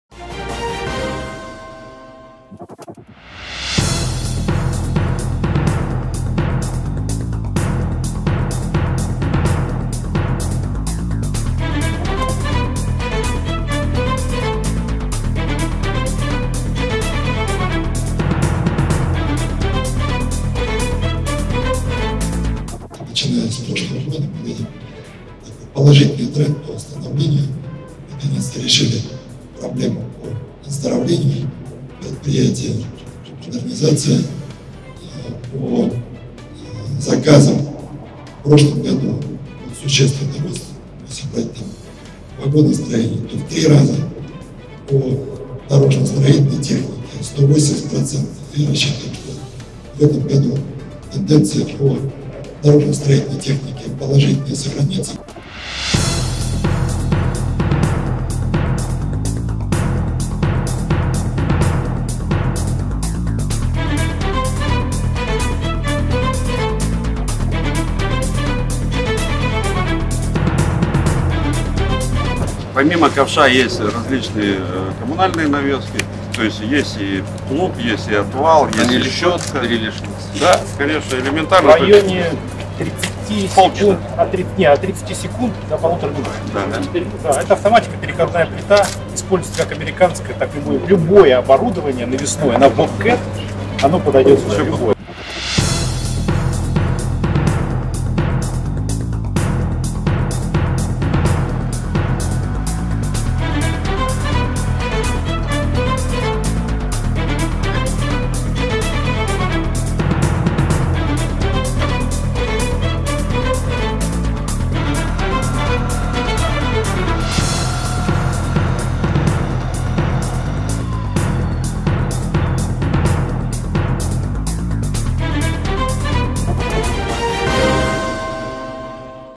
Денис Мантуров на Промтракторе
Визит на предприятие высокого гостя - повод для гордости.🔝
А внимание федерального министра - радость вдвойне.

Благодарим министра промышленности Дениса Мантурова за оценку нашей работы.